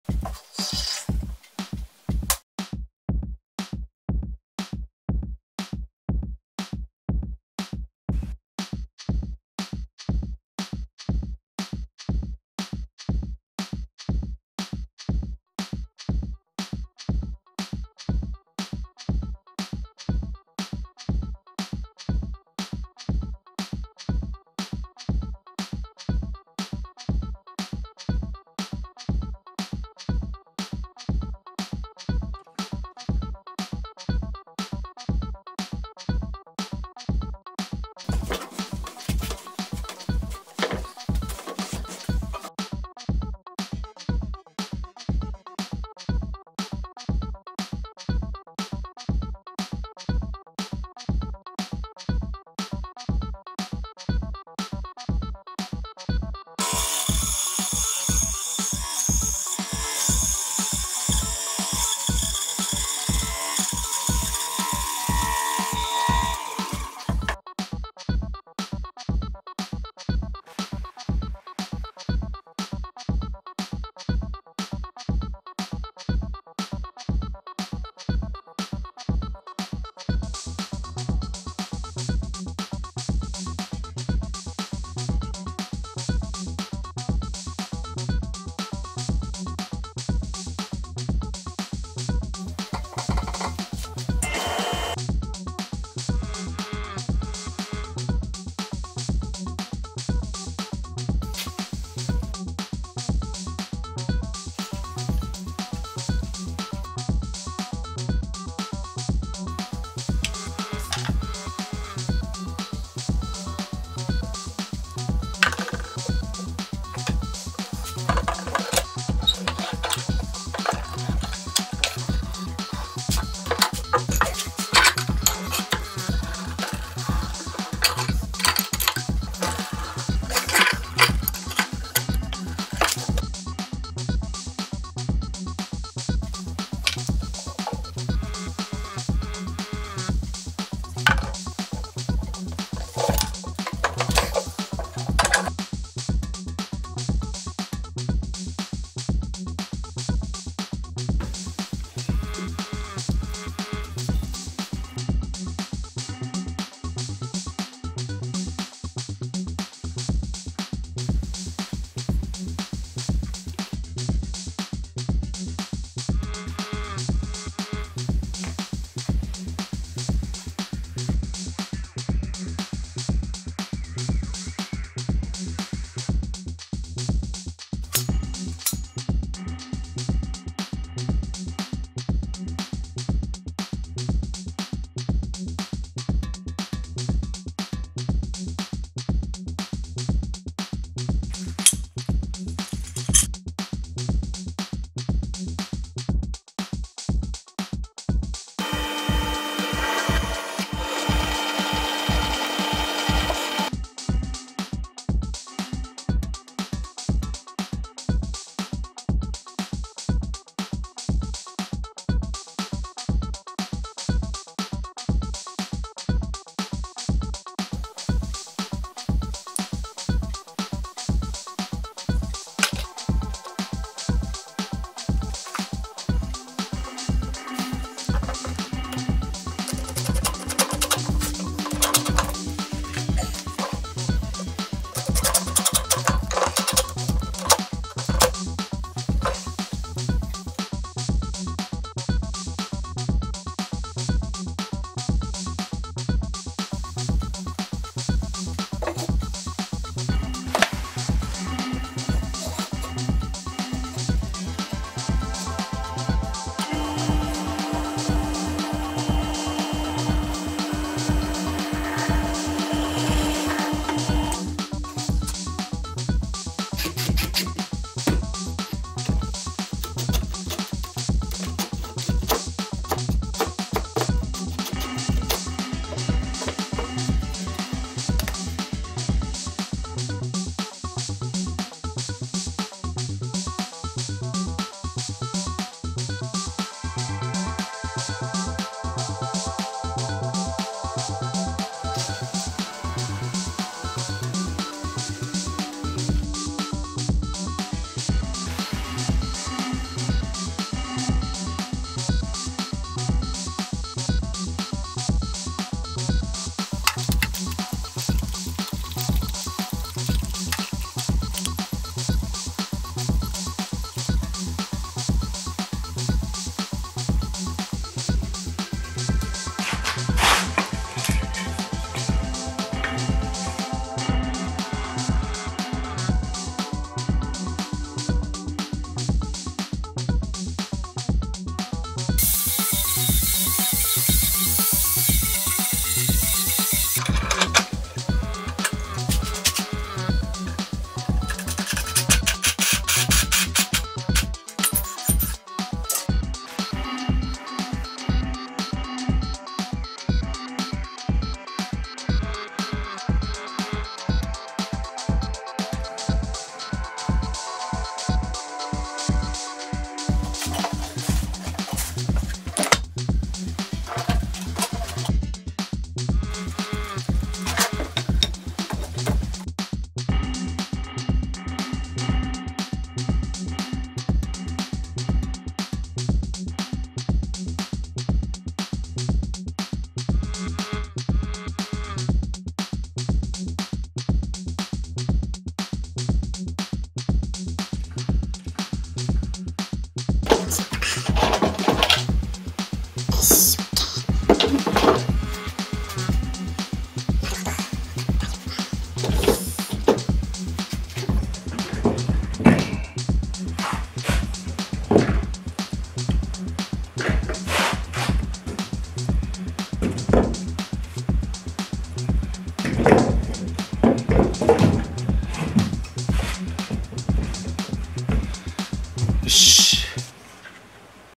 【DIY】long bar clamp.ロングバークランプ【自作】
The state which writes long bar clamp of length 1m by itself was made a video.
1mのロングバークランプを自作する動画です。